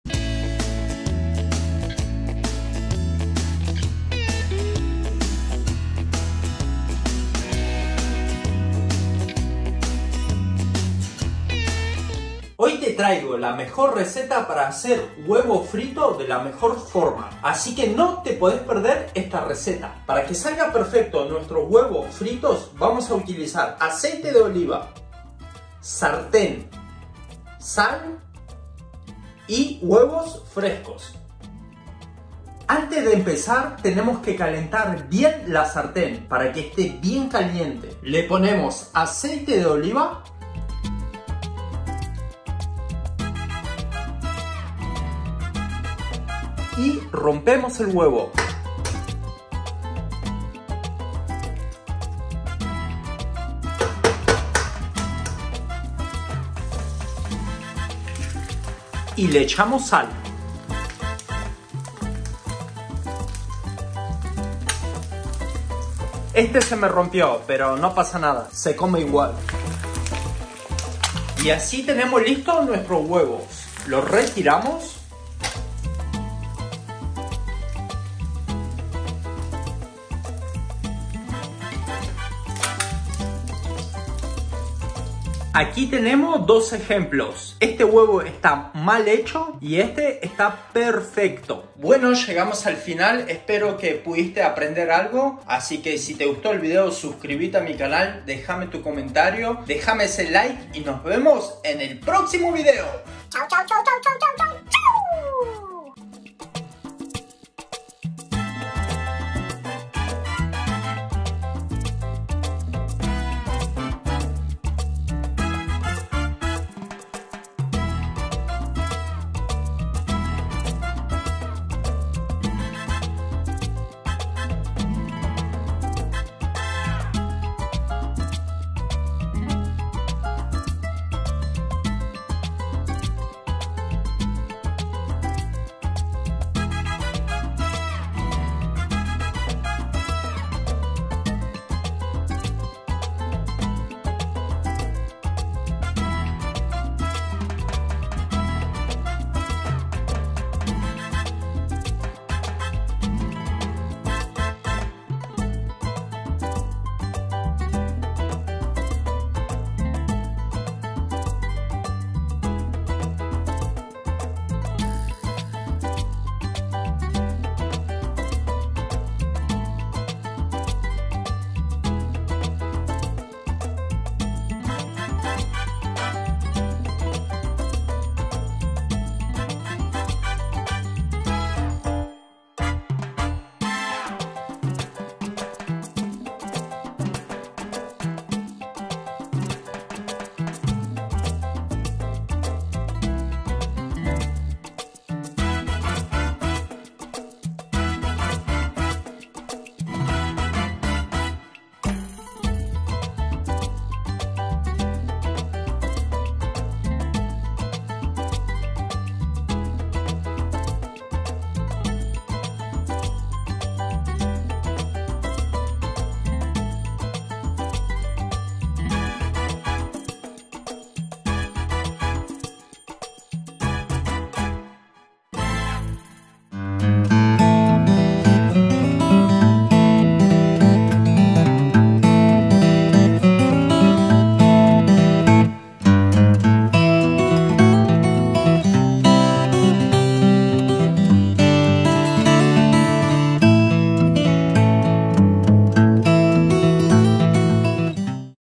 COMO HACER HUEVOS FRITOS DE LA MANERA CORRECTA - NIVEL DIOS - RECETA EN 4K
LAS MEJORES RECETAS DE MADE IN MISHA - FACIL Y BIEN RICO
SALUDOS -Misha.